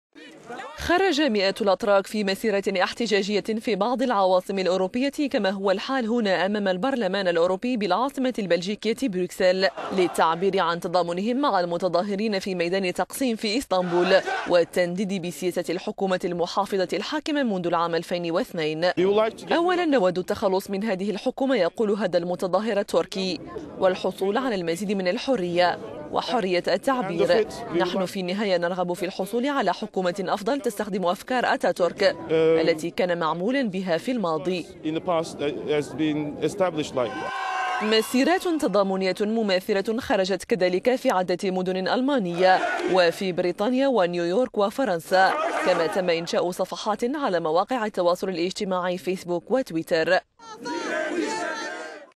مسيرات احتجاجية في أوروبا للتضامن مع متظاهري ميدان...
خرج مئات الأتراك في مسيرات احتجاجية في بعض العواصم الأوروبية كما هو الحال هنا أمام البرلمان الأوروبي بالعاصمة البلجيكية بروكسل، للتعبير عن تضامنهم مع المتظاهرين في ميدان تقسيم في اسطنبول،وغضبهم من استخدام الشرطة للعنف المفرط ضد المتظاهرين ،وللتنديد بسياسة الحكومة الاسلامية المحافظة الحاكمة منذ العام 2002.

 المحتجون رفعوا شعارات "كلنا ميدان تقسيم"،وكلنا سنقاوم معكم" وأردوغان ارحل"

يقول هذا المتظاهر التركي:"أولا نود التخلص من هذه الحكومة،والحصول على المزيد من الحرية،وحرية التعبير،نحن في النهاية نرغب  في الحصول على حكومة أفضل تستخدم افكار أتاتورك التي كان معمولا بها في الماضي."

مسيرات تضامنية مماثلة خرجت كذلك في عدة مدن ألمانية وفي بريطانيا و نيويورك وفرنسا.
كما تم إنشاء صفحات على مواقع التواصل الاجتماعي فيسبوك وتويتر.

    الاحتجاجات تفجرت بسبب خطط الحكومة الرامية لتحويل حديقة جيزي التي تضم ستمائة  شجرة وترجع للعهد العثماني إلي منطقة تجارية في ميدان تقسيم في اسطنبول،قبل أن تتحول الاحتجاجات الى حركة احتجاج سياسي عنيفة ضد مظاهرات مناهضة لحكومة رئيس الوزراء التركي رجب طيب أردوغان.